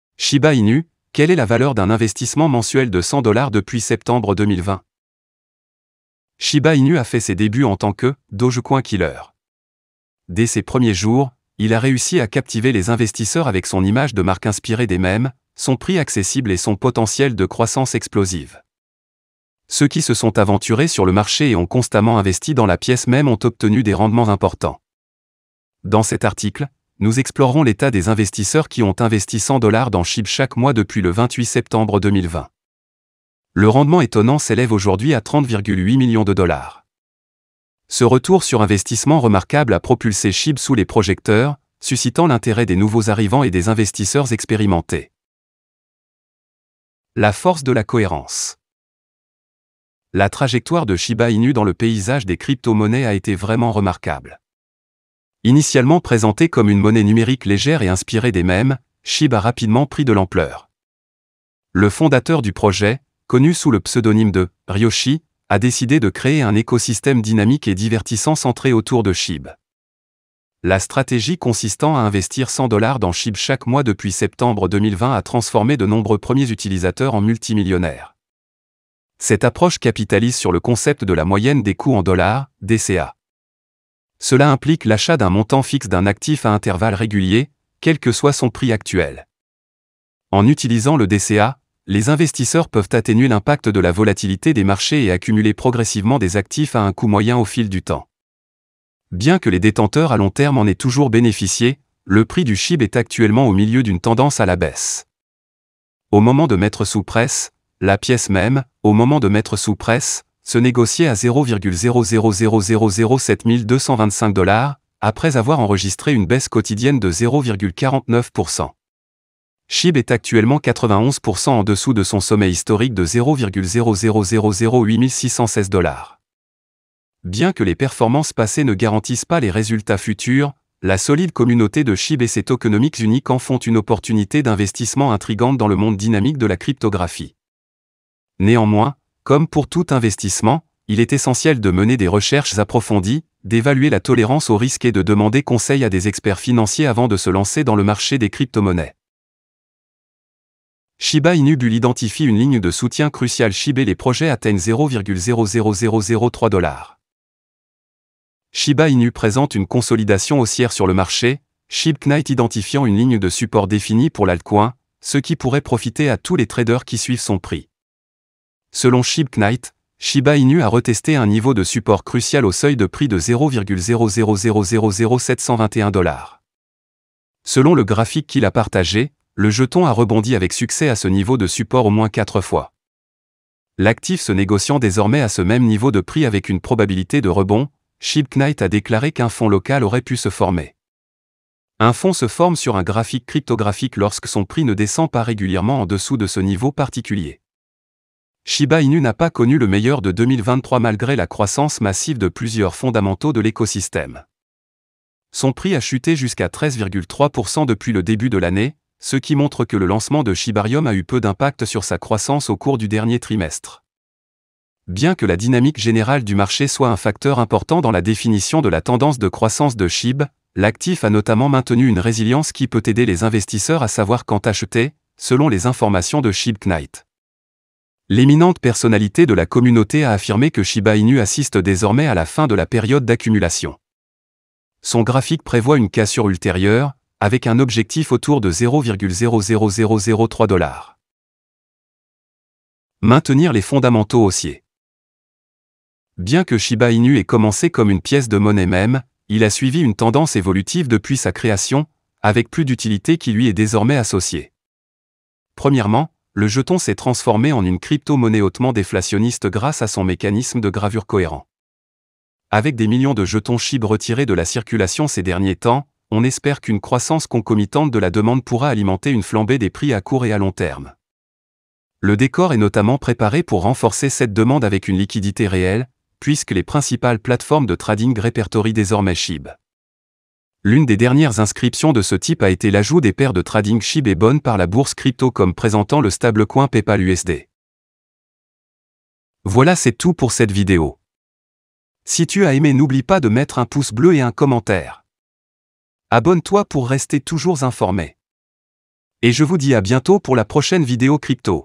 🚨 SHIBA INU : INVESTIR 100$ par MOIS est une bonne idée ? 🚀 LE BULL MARKET du SHIB ? 🔥 (actu crypto)
(+20% bonus)